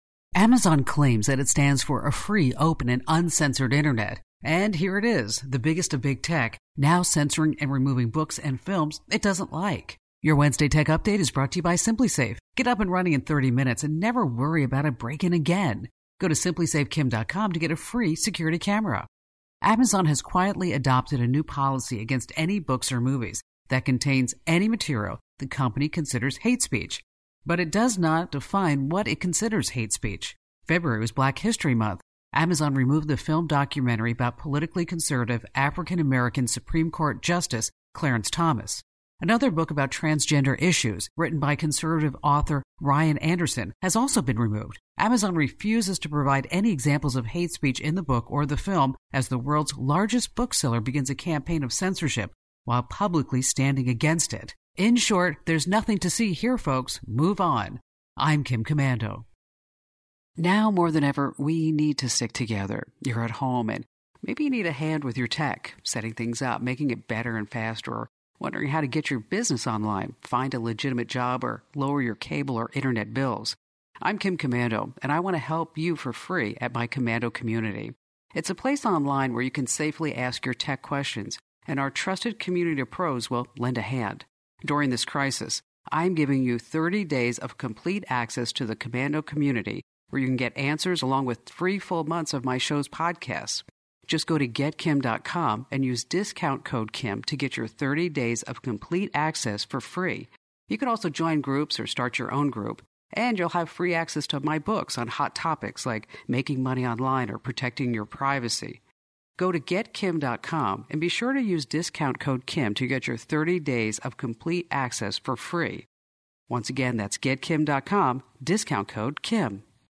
Amazon's censorship problem
Amazon claims that it stands for a free, open and uncensored Internet. Yet here it is, the biggest of Big Tech, now censoring and removing books and films it doesn’t like.